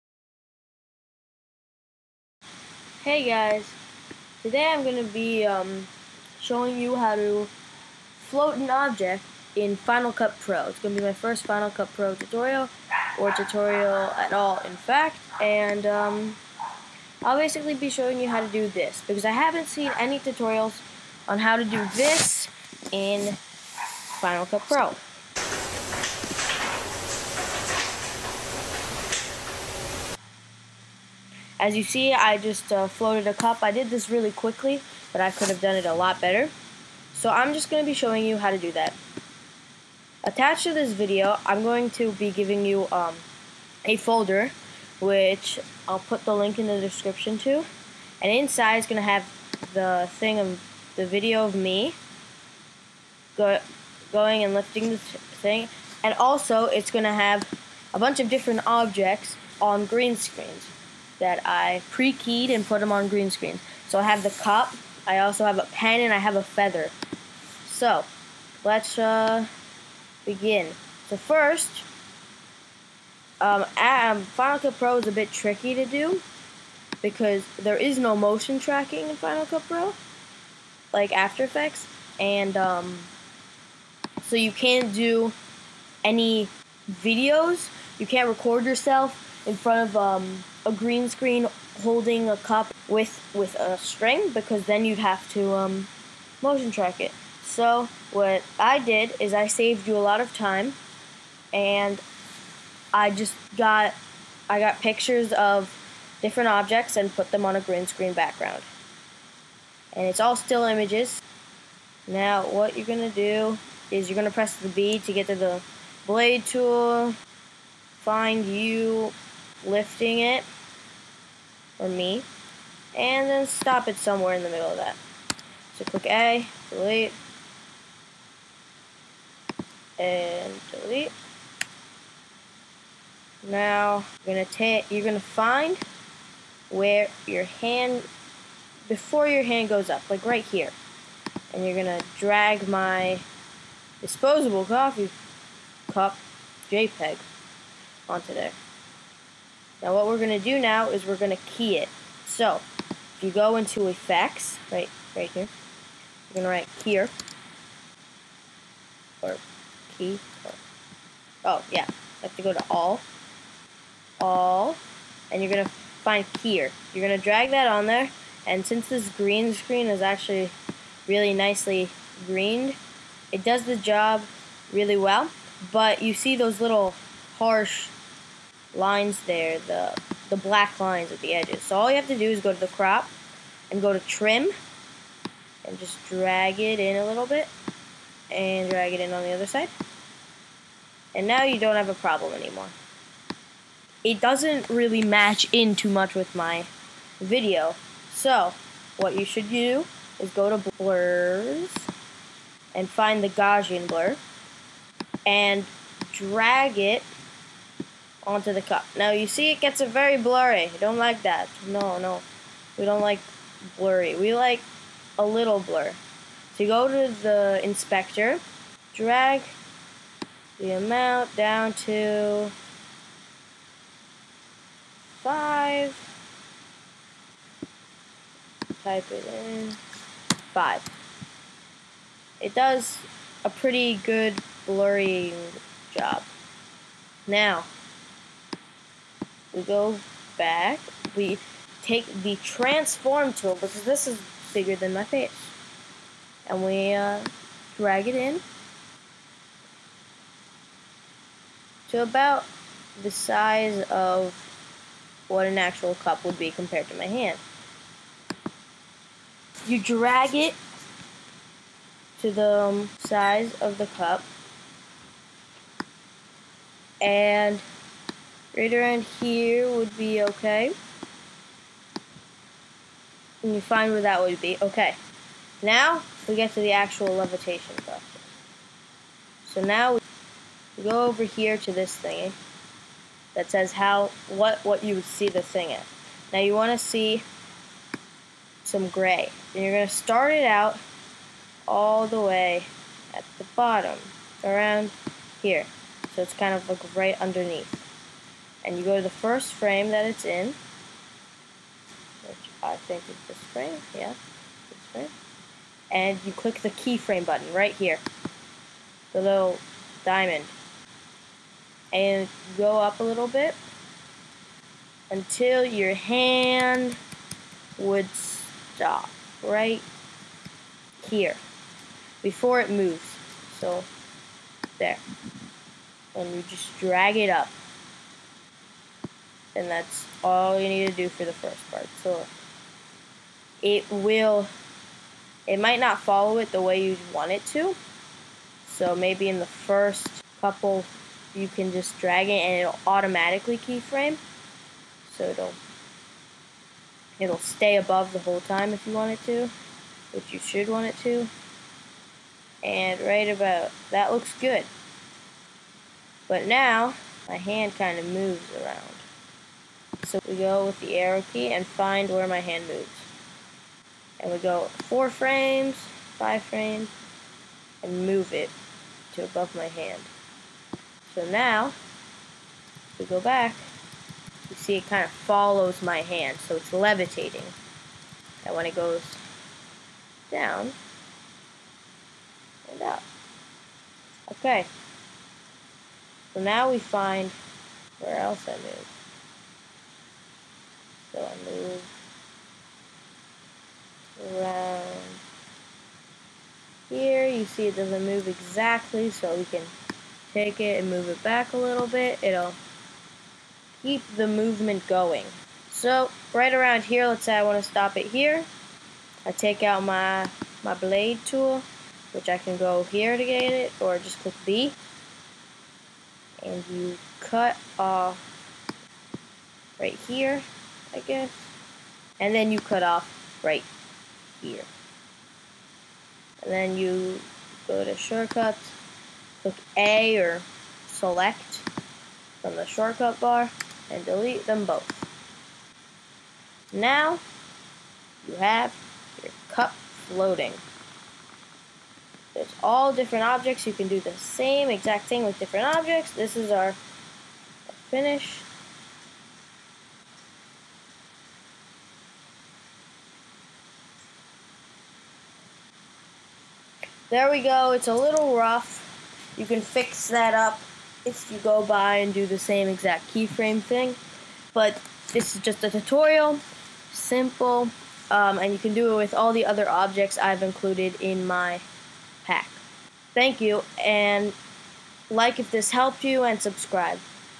How To Levitate Objects In Final Cut Pro X - FCPX Tutorial
This is my first tutorial and I hear that a lot of you have final cut pro X so I decide to make a tutorial for Final Cut Pro X